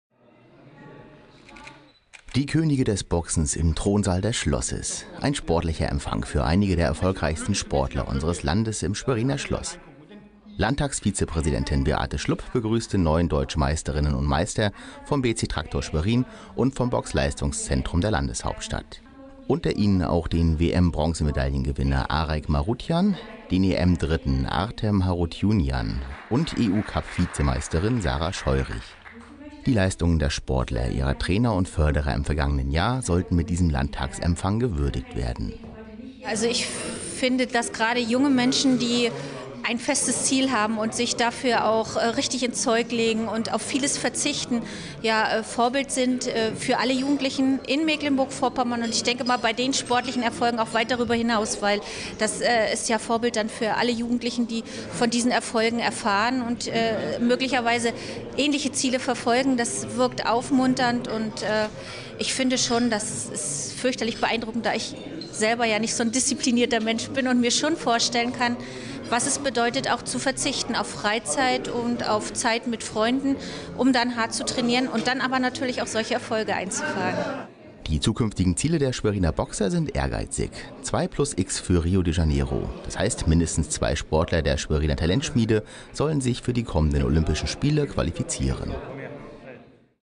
Erfolgreiche Boxer
Empfang für Deutsche Meister im Landtag